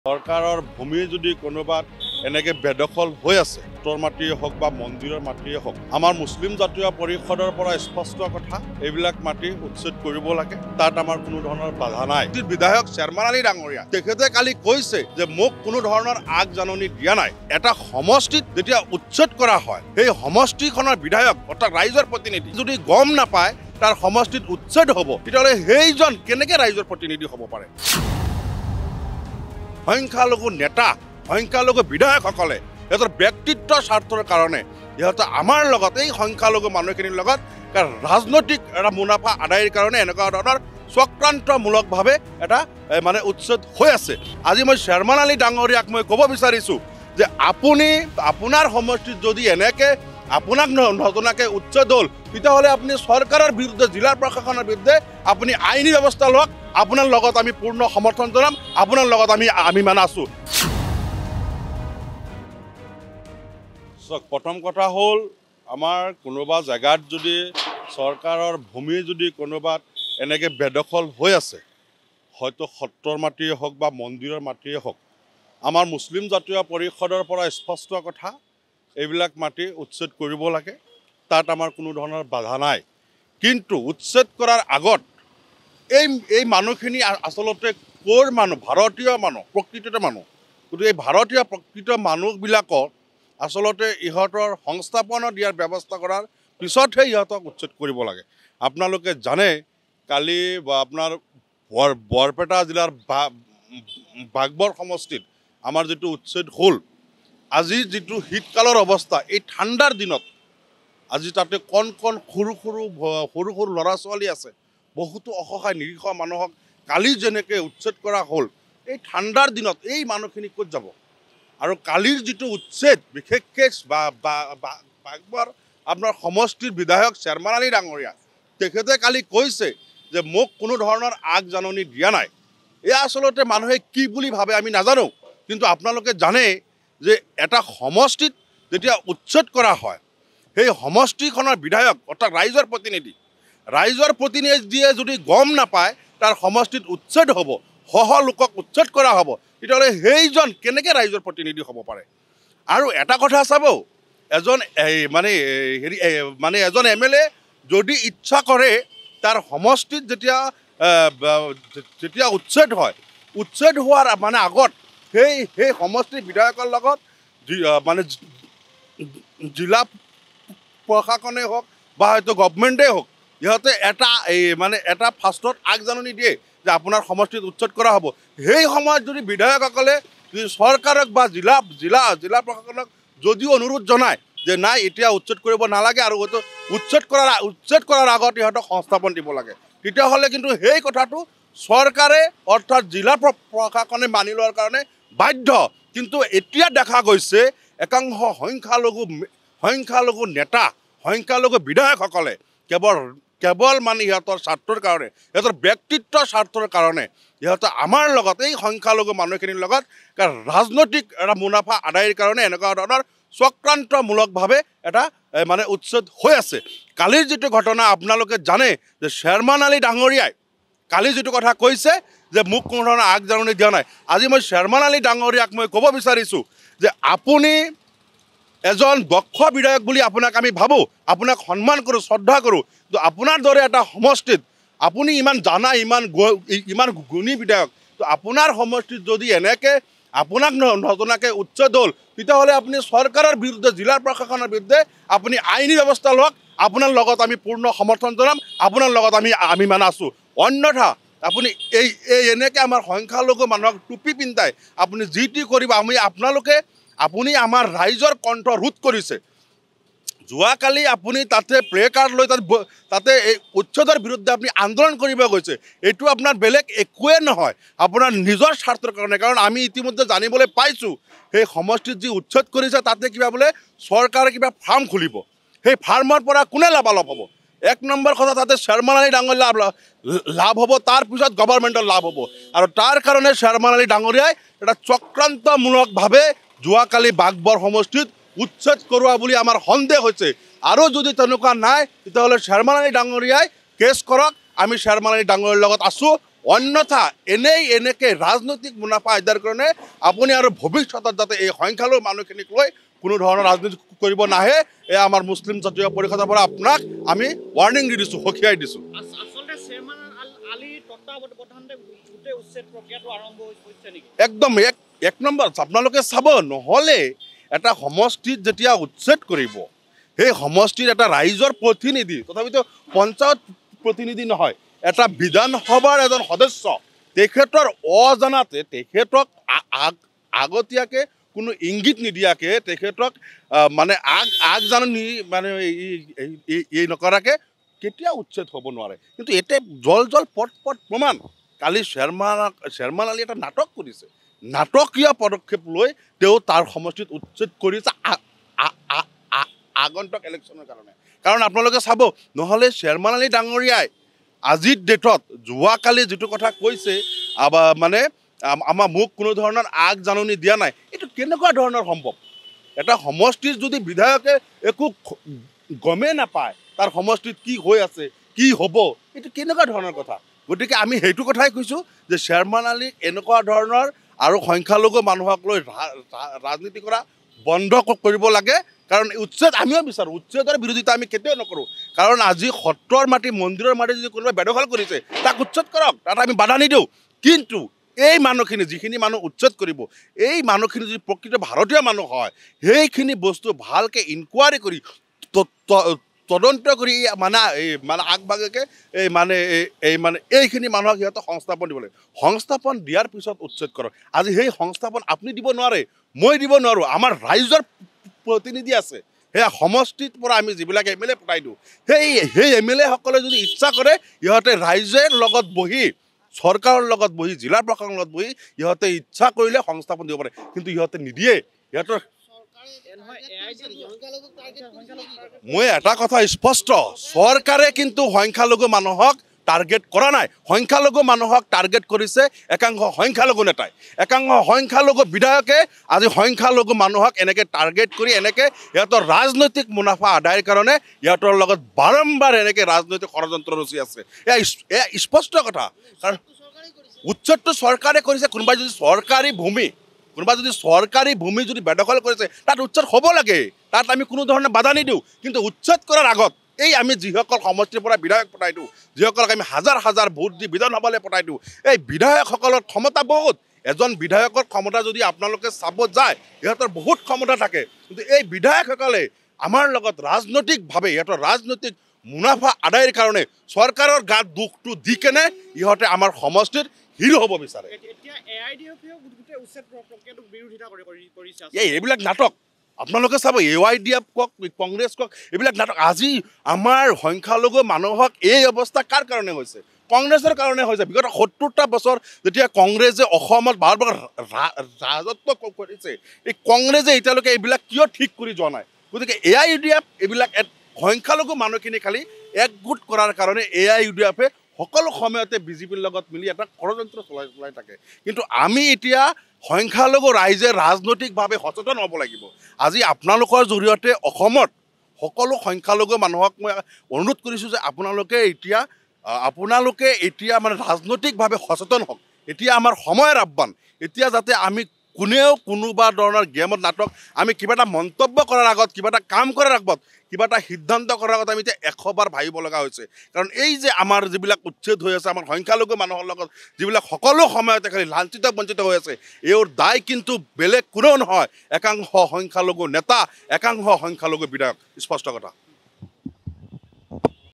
'সংখ্যালঘু নেতা, বিধায়কসকলে ব্যক্তিগত স্বাৰ্থৰ বাবে সংখ্যালঘু লোকক উচ্ছেদ কৰি আহিছে।'
📌'সংখ্যালঘু নেতা, সংখ্যালঘু বিধায়ক সকলে ব্যক্তিগত স্বাৰ্থৰ বাবে সংখ্যালঘু লোকক উচ্ছেদ কৰি আহিছে।'
📌'শ্বেৰমান আলীয়ে সংখ্যালঘু লোকৰ কণ্ঠ ৰোধ কৰাৰ চেষ্টা চলাই আহিছে '
 📌'এই পচন্দ শীতৰ মাজত উচ্ছেদিতসকল এতিয়া ক'লৈ যাব?  '
📌উচ্ছেদৰ সমৰ্থনত এইবাৰ খোদ মুছলিম জাতীয় পৰিষদ